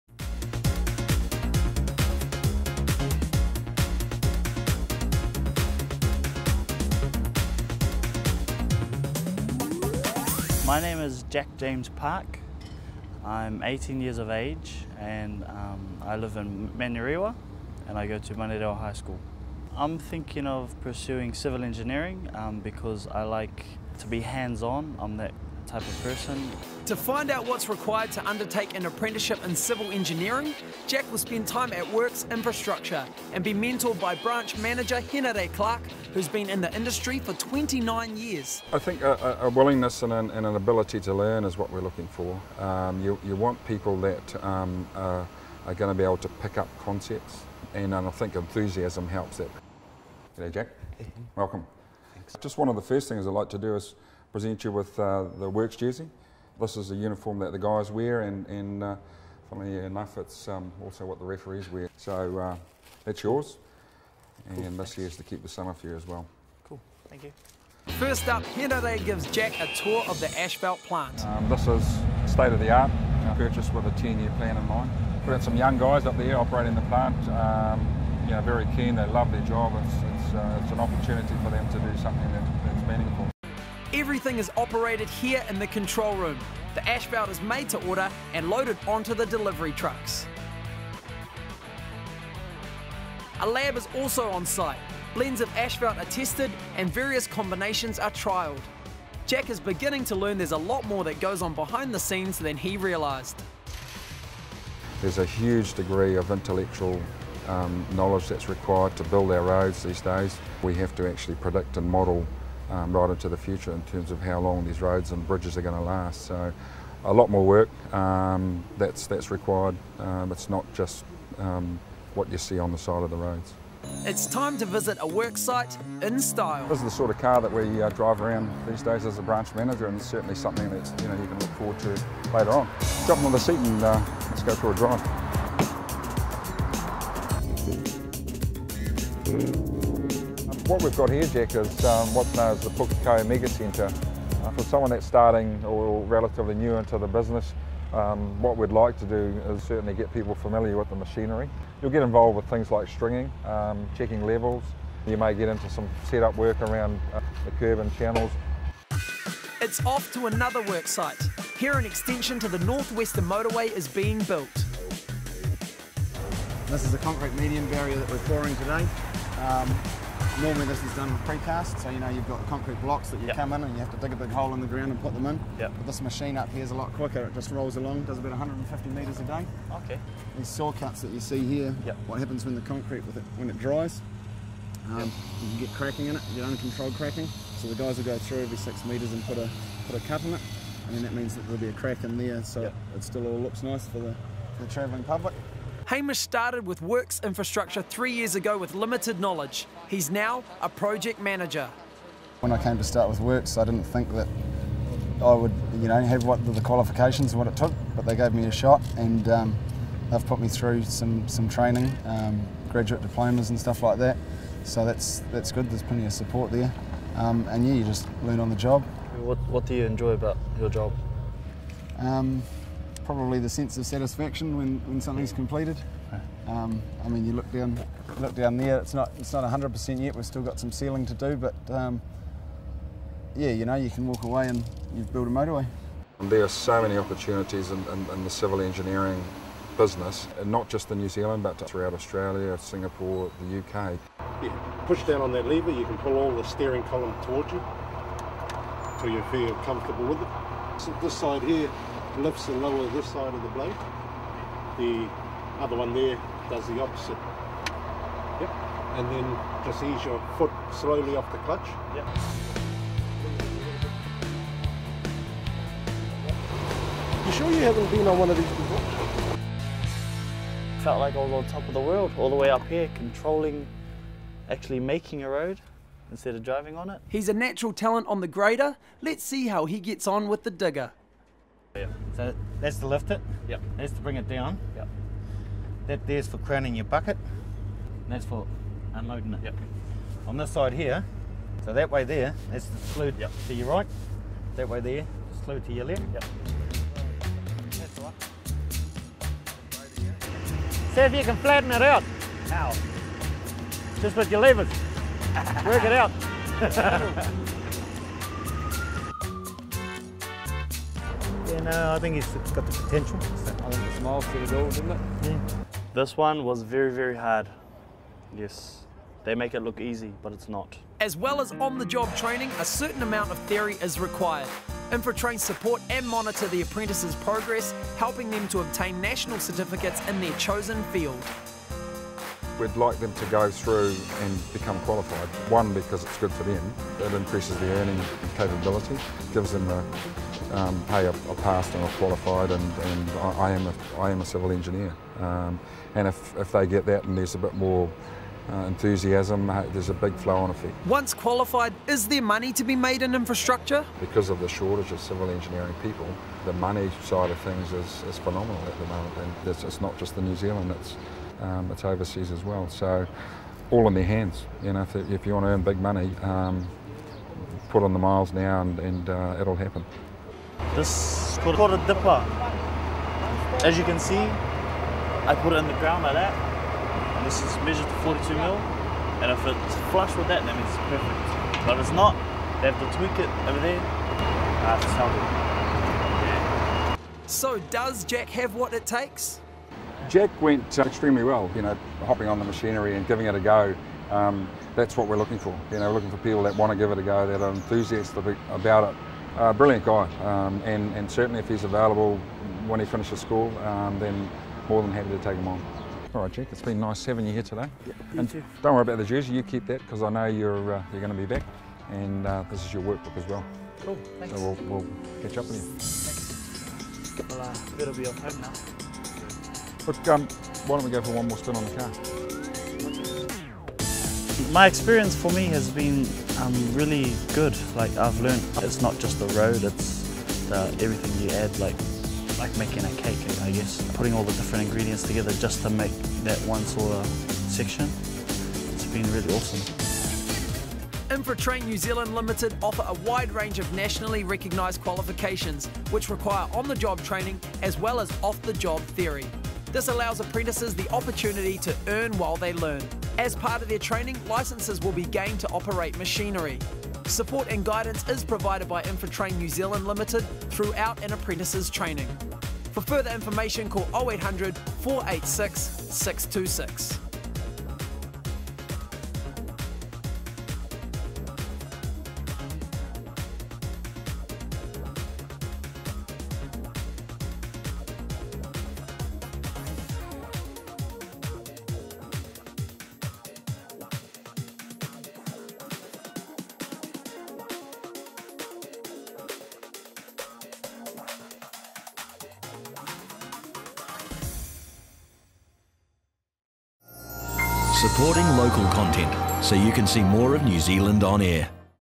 A Career in Civil Construction (JTJS12007)
Jack Parks (18) of Manurewa High School is first up this week and he thinks the hands on nature of civil engineering might be just the job for him. With an up close look at what goes on with the making of a major motorway, he finds out the skills he could learn on the job could open up other opportunities internationally.
Jack has no qualms about getting in there and getting involved and realises a role with Works Infrastructure is a bit like a cake - lots of ingredients are required for a good result!